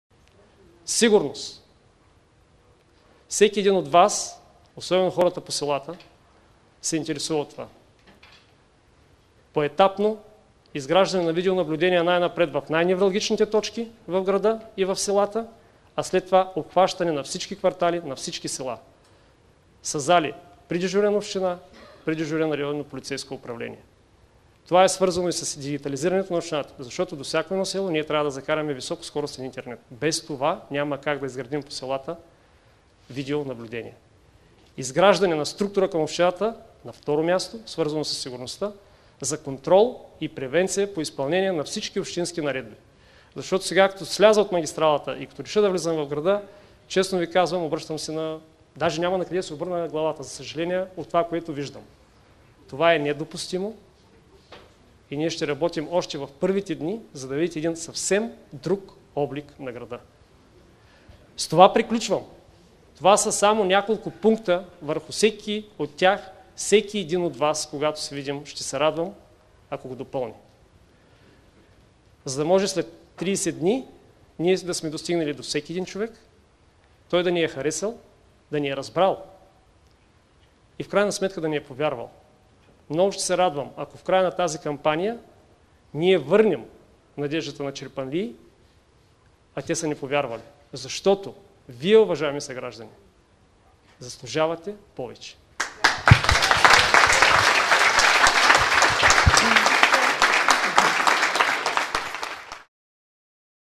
Ивайло Крачолов - Сигурност в Чирпан и в селата от общината
Ивайло Крачолов, кандидат за кмет на Чирпан:
Чирпан заслужава повече
„Приемам с огромна отговорност това, което ни предстои по време на предизборната кампания, а след това убеден съм, и по време на мандата. Извървяхме дълъг път през това лято, проведохме много срещи, много открити и почтени разговори, за да стигнем до тук, да имам до себе си едни прекрасни хора, които днес са кандидати за общински съветници”. С тези думи кандидатът за кмет на Чирпан Ивайло Крачолов се обърна към жителите на общината. Ивайло Крачолов е издигнат от най-голямата в историята на града коалиция.
„Направихме нещо уникално. Обединихме се партии от крайно дясно в своята идеологии, през център до ляво, но обединението е в името на Чирпан” – каза още Ивайло Крачолов.
Той сподели, че през последните години е живял извън Чирпан, но никога не е късал връзката си с града. „Посетих много общини, видях добри практики и много хора ми казаха, че трябва да се върна и да помогна. Дадох сметка, че човек трябва да превъзмогне егоизма си и да придаде натрупания опит, за да бъде полезен” – добави кандидатът за кмет на Чирпан Ивайло Крачолов.
Той представи приоритетните сектори, в които ще работи, като призова съгражданите си да участват активно с идеи и предложения.
„Чирпан се намира на прекрасно място и ще става все по-атрактивен за инвеститори. Ще направим необходимо Общината да получи собствеността на бившето поделение. Този терен е атрактивен за бизнеса, а аз знам какво искат предприемачите, защото съм работил за голяма корпорация” – съобщи Ивайло Крачолов. Той добави, че за привличането на инвеститори е необходима добра инфраструктура и ако кандидатите на коалицията спечелят изборите, още в началото на следващата година ще започне обновяване на подземната инфраструктура и след това на улиците. Ще се въведе и поетапно видеонаблюдение в града, ще се изградят спортни площадки, ще бъде направен основен ремонт на читалищната зала.
„Много ще се радвам ако в края на предизборната кампания върнем надеждата на хората и те да ни повярват” – каза още Ивайло Крачолов.

ilko gruev